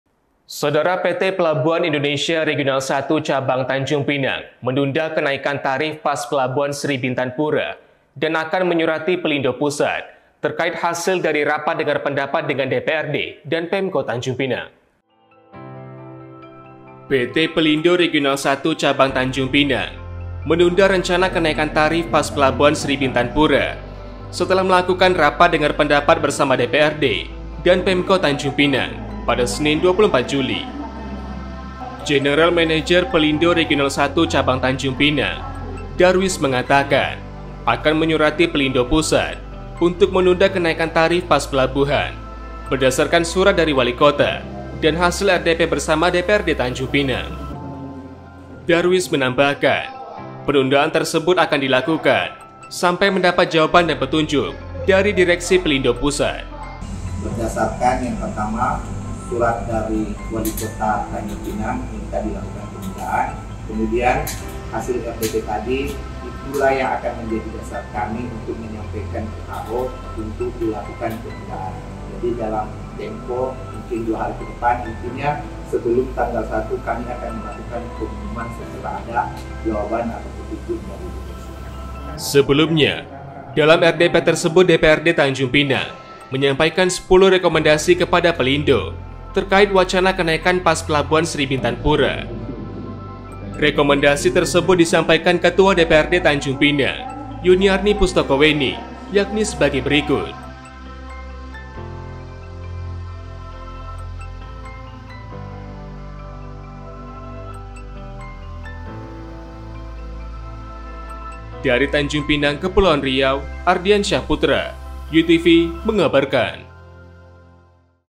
PELINDO TUNDA KENAIKAN PAS PELABUHAN, TUNGGU PETUNJUK PELINDO PUSAT | U-NEWS
TANJUNGPINANG - PT Pelabuhan Indonesia Regional I Cabang Tanjungpinang akan menunda kenaikan tarif pas Pelabuhan Sri Bintan Pura, dan akan menyurati Pelindo pusat terkait hasil dari rapat dengar pendapat dengan DPRD dan Pemerintah Kota Tanjungpinang.

Berikut Liputannya.